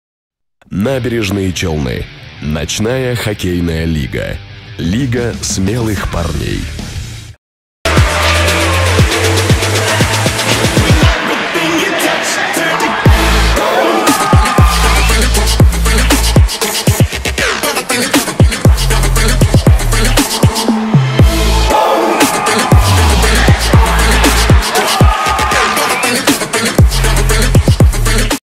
Гол №2 Дмитрий Солдатенко (ЮНОСТЬ)
Хоккейный турнир Л30+ Набережные Челны-2017